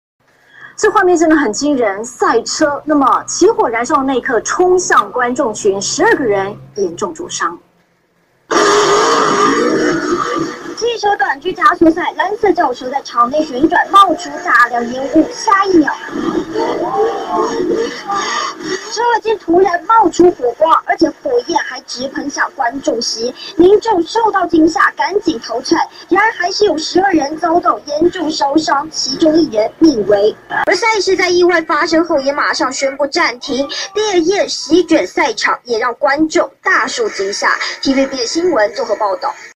賽車起火燒觀眾席 12人灼傷(聲音)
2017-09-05
[頻道]TVBS-N(55台)